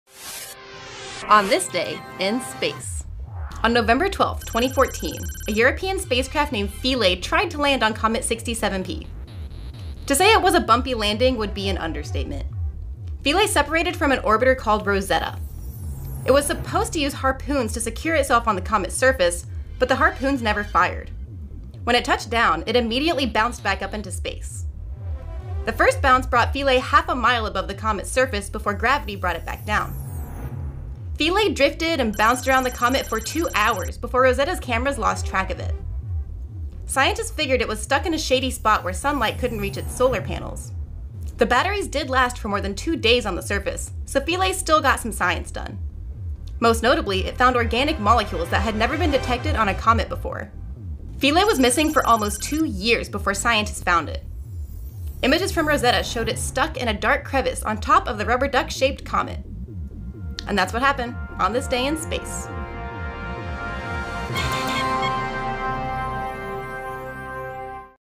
OTD in Space - Nov. 12: Philae Spacecraft Gets Lost on Comet
On November 12, 2014, a European spacecraft named Philae tried to land on Comet 67P. To say it was a bumpy landing would be an understatement. Philae separated from an orbiter called Rosetta. It was supposed to use harpoons to secure itself on to the comet surface, but the harpoons never fired. When it touched down, it immediately bounced back up into space. The first bounce brought Philae half a mile above the comet's surface before gravity brought it back down. Philae drifted and bounced around the comet for two hours before Rosetta's cameras lost track of it. Scientists figured it was stuck in a shady spot where sunlight couldn't reach its solar panels. The batteries did last for more than two days on the surface, so Philae still got some science done. Most notably, it found organic molecules that had never been detected on a comet before. Philae was missing for almost two years before scientists found it. Images from Rosetta showed it stuck in a dark crevice on top of the rubber duck-shaped comet.